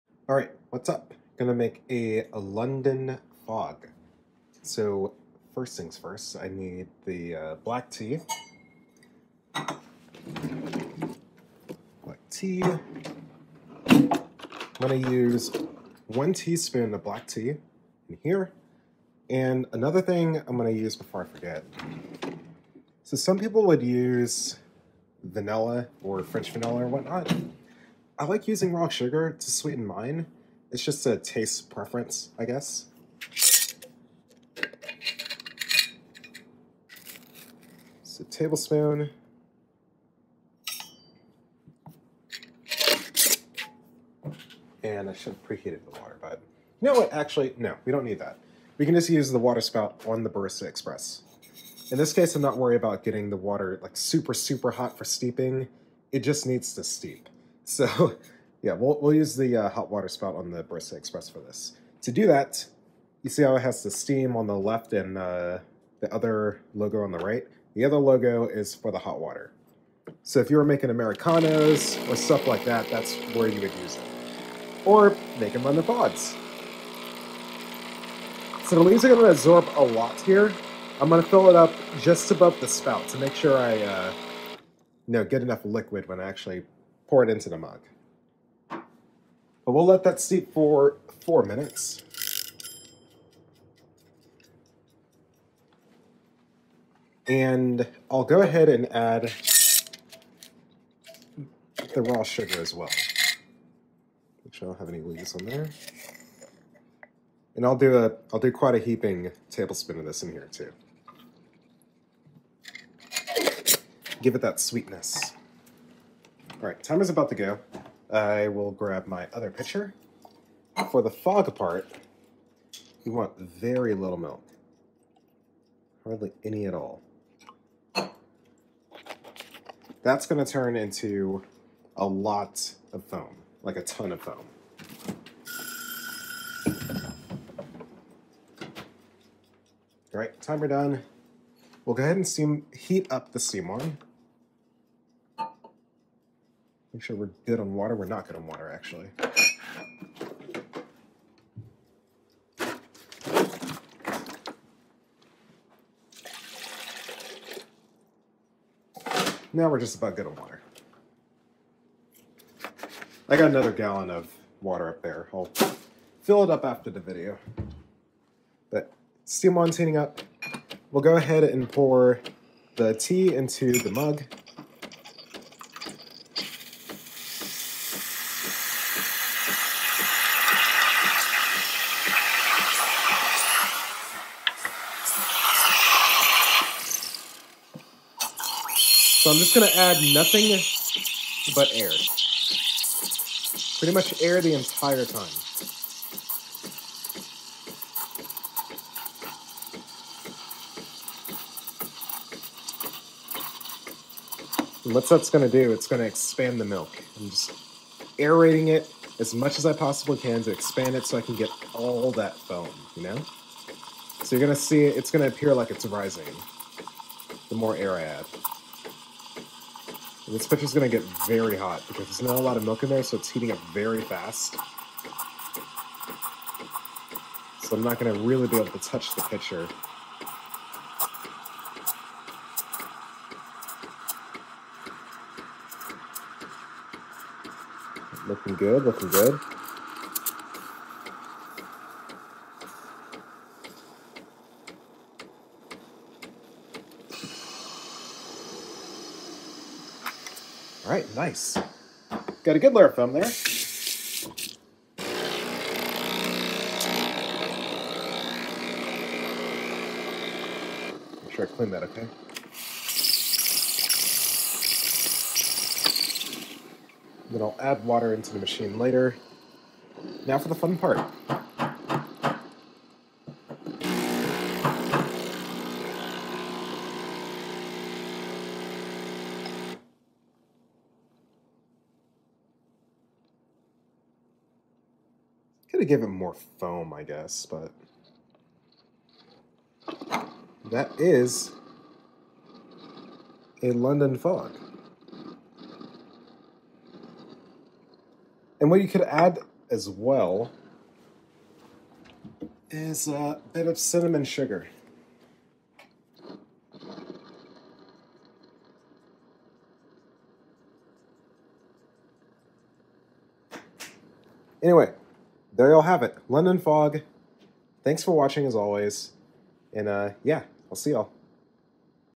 How To Make a London Fog with Breville Barista Express POV
Here's a POV style video on making London Fogs with a little help from the Breville Barista Express espresso machine.